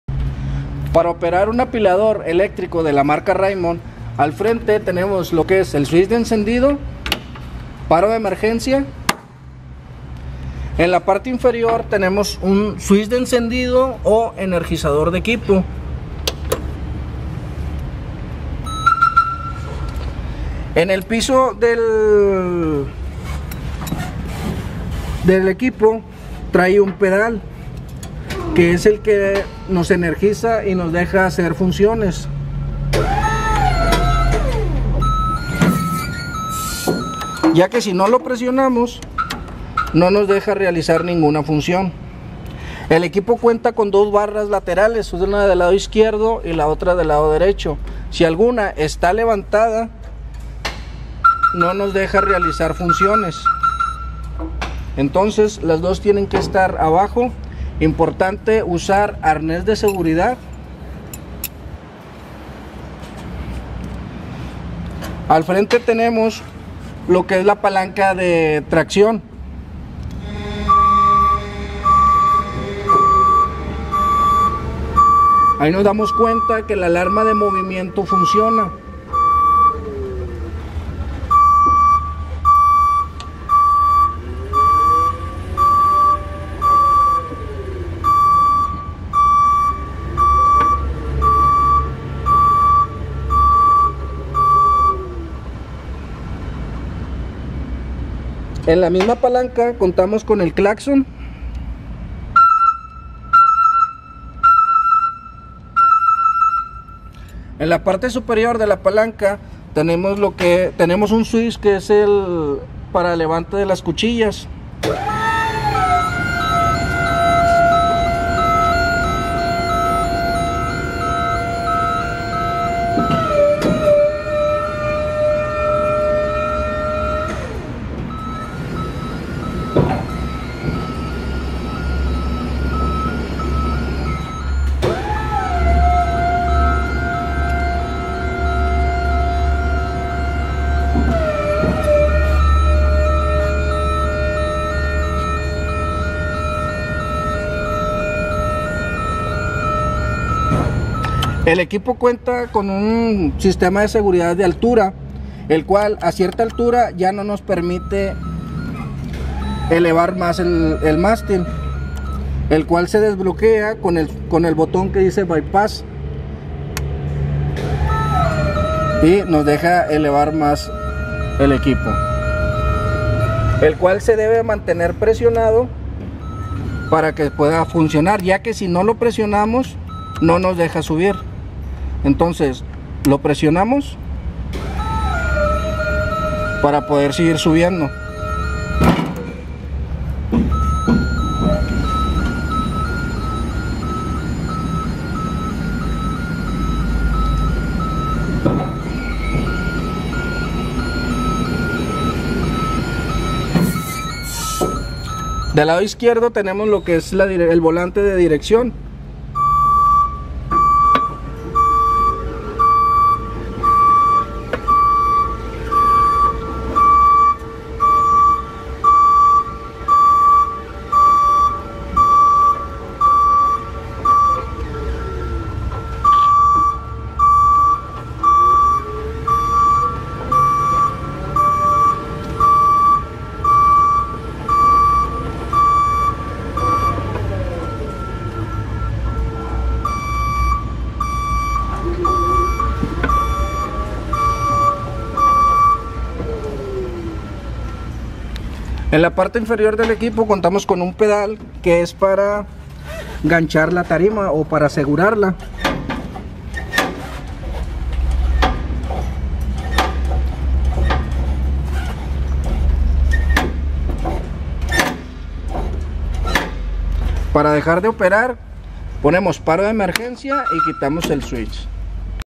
Cómo Operar o Manejar un Apilador Eléctrico Marca RAYMOND Modelo 520-OPC30TT de 3000 Libras
Transformadores de todos tipos y potencias, así como material eléctrico y accesorios para alta tensión